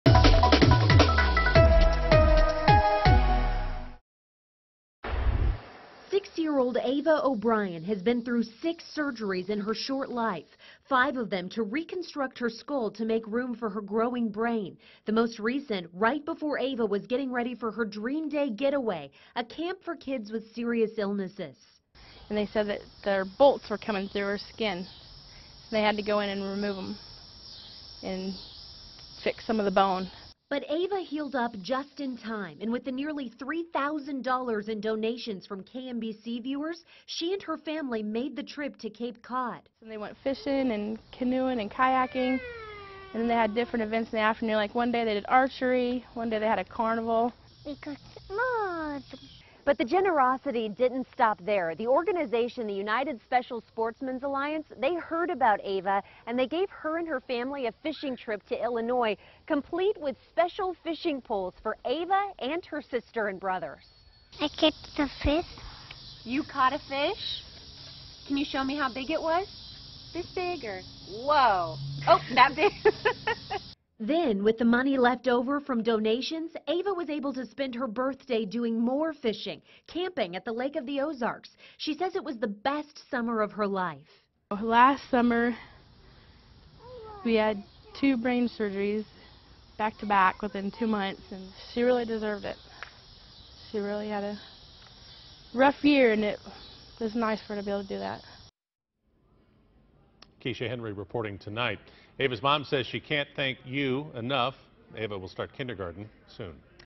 Generous gifts help KC girl enjoy spectacular summer
The family of a little girl who just returned from a camp for children with life-threatening medical conditions is expressing gratitude to the public for giving her the best summer of her life.